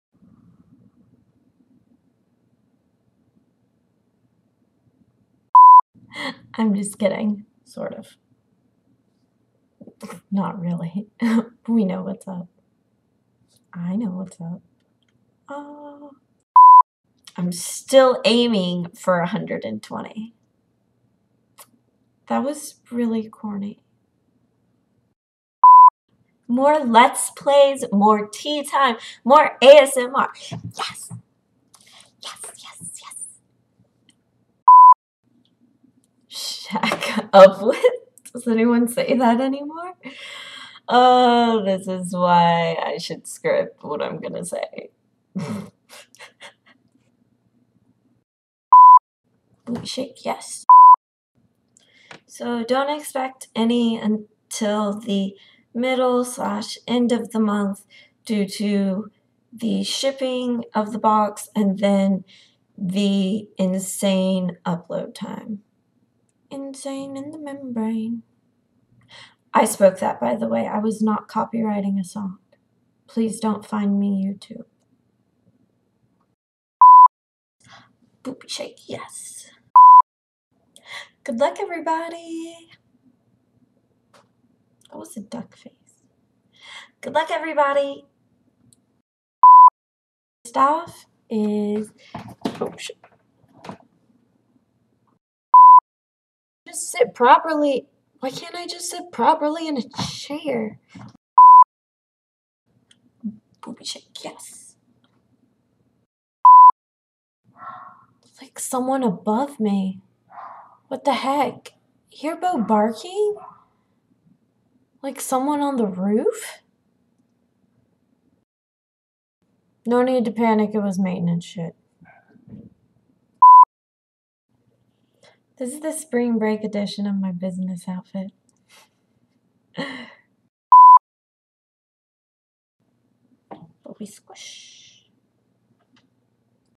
Tea Time with Binques ~ BLOOPERS #1
Bloopers.
Boopers.
Boop.
Boob.

**Short video of some of the funny and odd things that happen while filming Tea Time with Binques. Since the Tea Time episode was so long I decided to make this a separate video instead of adding them to the end.**